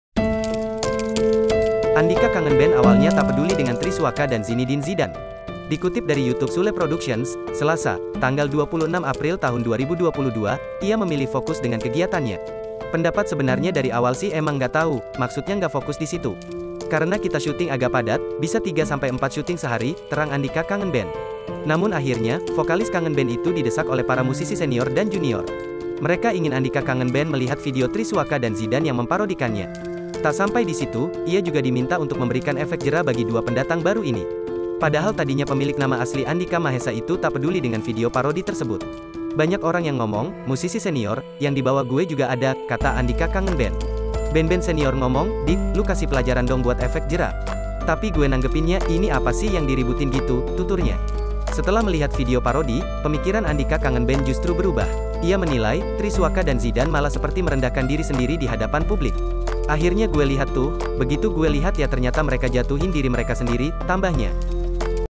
Andika Ngaku Didesak Agar Beri Efek Jera Tri Suaka dan Zidan
Andika | Andika Dapat Desakan | Andika Beri Efek Jera | Berita Hiburan | Berita Selebritis
TRIBUNLAMPUNG - Andika Kangen Band awalnya tak peduli dengan Tri Suaka dan Zinidin Zidan.
Dikutip dari YouTube Sule Productions, Selasa (26/4/2022), ia memilih fokus dengan kegiatannya.
"Pendapat sebenarnya dari awal sih emang nggak tahu, maksudnya nggak fokus di situ."
"Karena kita syuting agak padat, bisa tiga sampai empat syuting sehari," terang Andika Kangen Band.
Namun akhirnya, vokalis Kangen Band itu didesak oleh para musisi senior dan junior.
Mereka ingin Andika Kangen Band melihat video Tri Suaka dan Zidan yang memparodikannya.
Tak sampai di situ, ia juga diminta untuk memberikan efek jera bagi dua pendatang baru ini.
Padahal tadinya pemilik nama asli Andika Mahesa itu tak peduli dengan video parodi tersebut.
"Banyak orang yang ngomong, musisi senior, yang di bawah gue juga ada," kata Andika Kangen Band.
"Band-band senior ngomong 'dik, lu kasih pelajaran dong buat efek jera'."
"Tapi gue nanggepinnya 'ini apa sih yang diributin' gitu," tuturnya.
Setelah melihat video parodi, pemikiran Andika Kangen Band justru berubah.
Ia menilai, Tri Suaka dan Zidan malah seperti merendahkan diri sendiri di hadapan publik.
"Akhirnya gue lihat tuh, begitu gue lihat ya ternyata mereka njatuhin diri mereka sendiri," tambahnya.
Kendati demikian, Andika Kangen Band tetap bersikap bijak dengan memberikan doa baik.
"Dan gue bicara sama temen-temen, mari kita doain yang kaya gitu semoga sadar."
"Mereka 'kan belum sadar kalau mereka ini jadi publik figur," jelas Andika Kangen Band.
Lanjut, ia juga ogah terlalu fokus pada masalah dengan Zidan dan Tri Suaka ini.